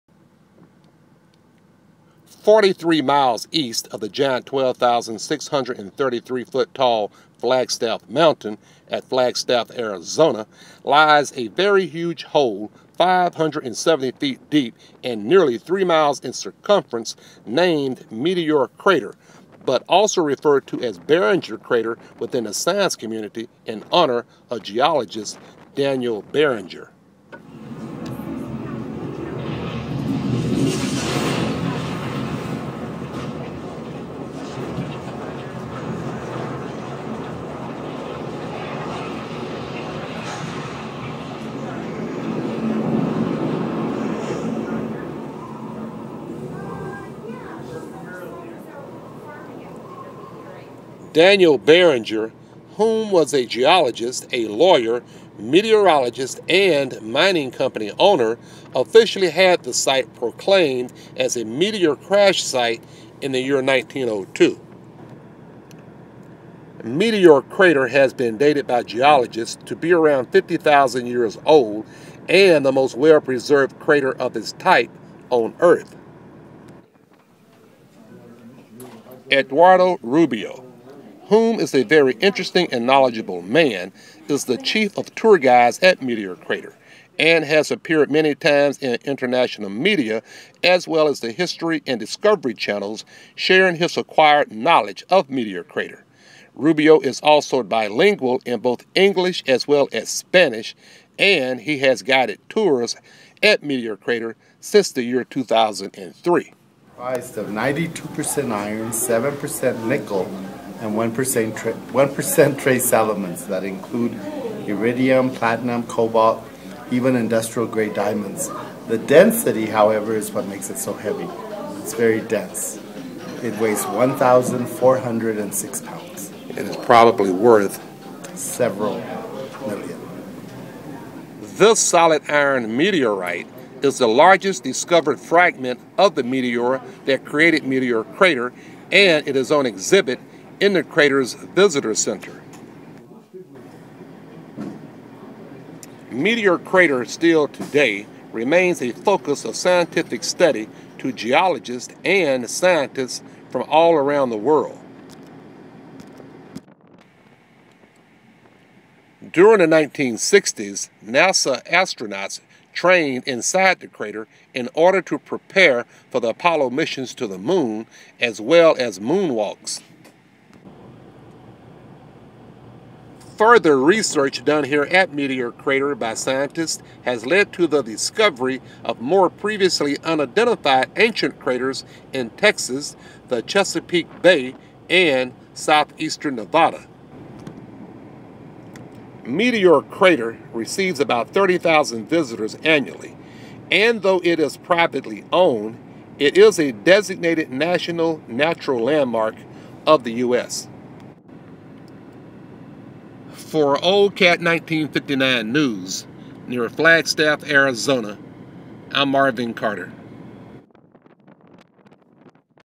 METEOR CRATER - Flagstaff Arizona
Forty-three miles east of the giant 12,633 foot tall Flagstaff Mountain at Flagstaff Arizona lies a very huge hole, 570 feet deep, and nearly three miles in circumference, named Meteor Crater. But also referred to as  Barringer Crater within the science community, in honor of geologist Daniel Barringer.
Barringer whom  was a geologist, a lawyer, meteorologist and mining company owner, officially had this site proclaimed as a meteor crash site in the year 1902.